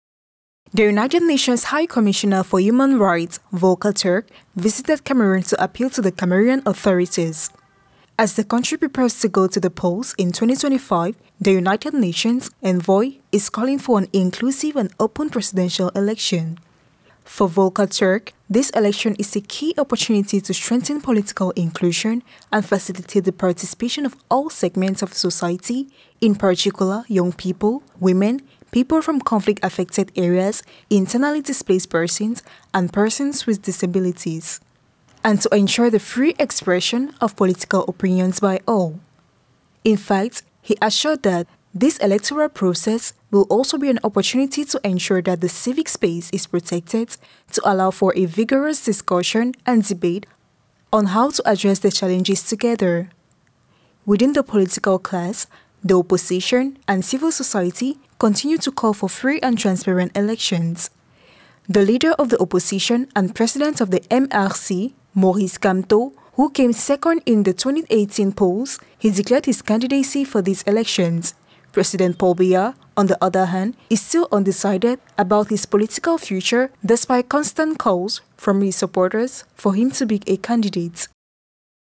CAMEROON: THE UN CALLS FOR INCLUSIVE VOTING DURING 2025 PRESIDENTIAL ELECTIONS
In 2025, Cameroonians will go to the polls to elect their new president. With just a few months to go to the polls, the UN's human rights chief has called for an open and fair ballot.